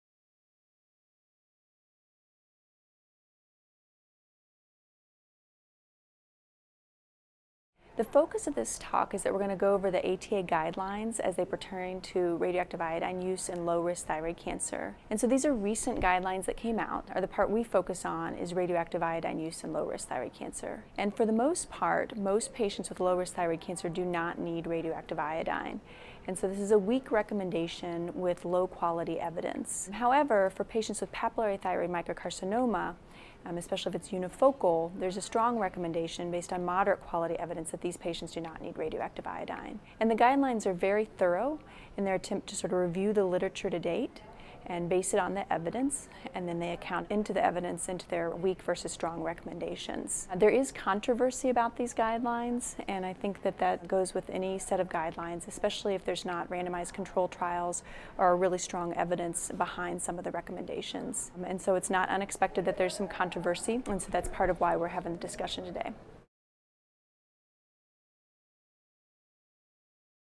Radioactive Iodine: To Treat or Not To Treat Low Risk Thyroid Cancer
Dr. Megan Haymart discusses her presentation at the Endo 2016 meeting in Boston.